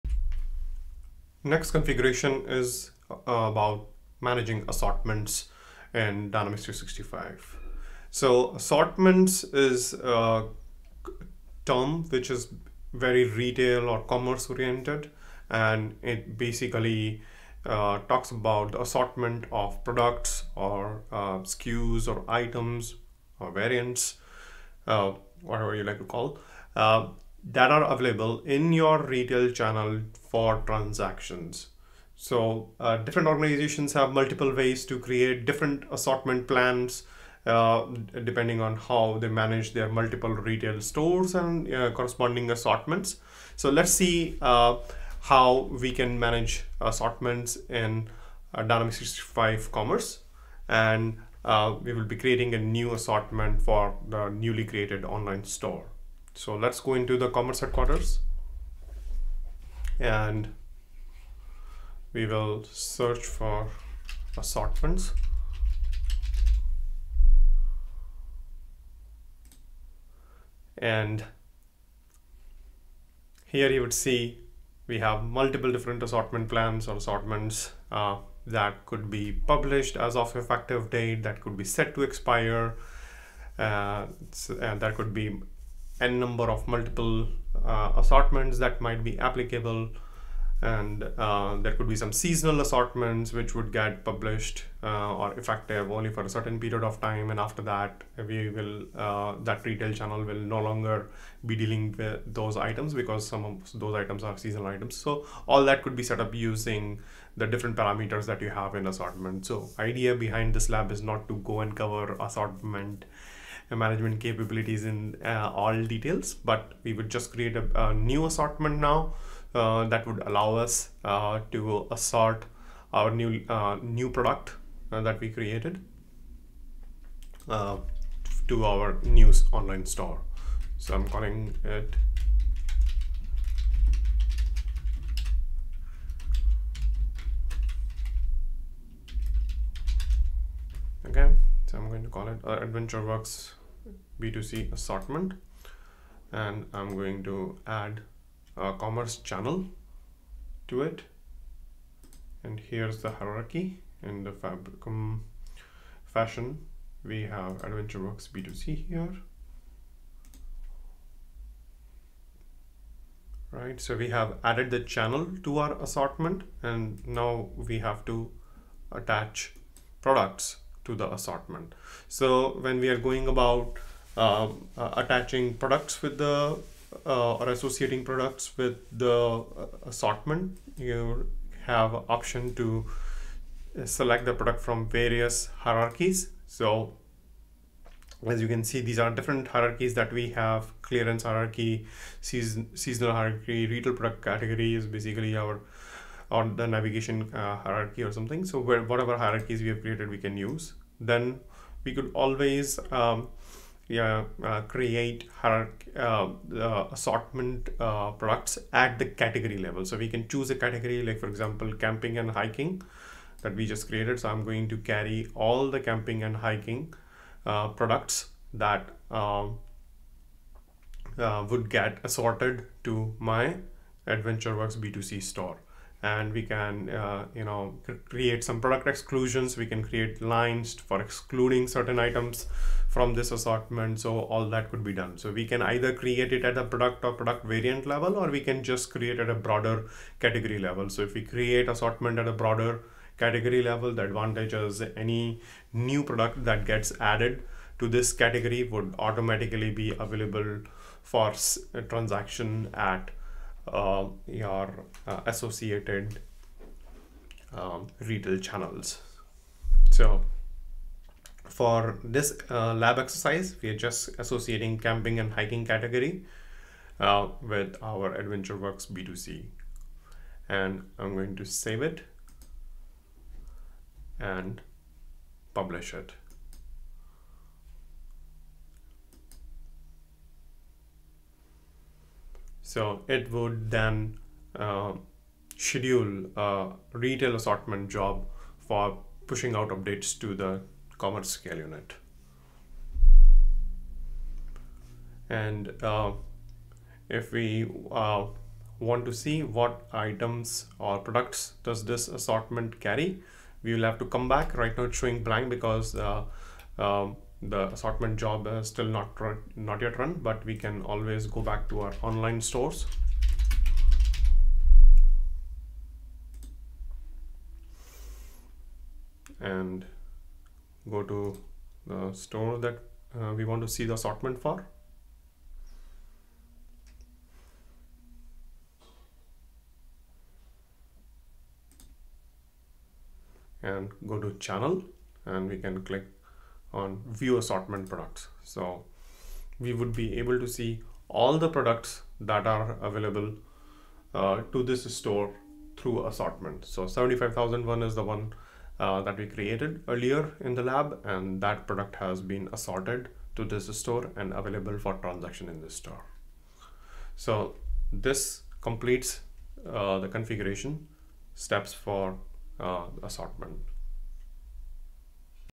D365 Commerce Academy - Assortments
Commerce Academy Lab 09 - Assortment management in Commerce. Create and publish a new assortment for new online store.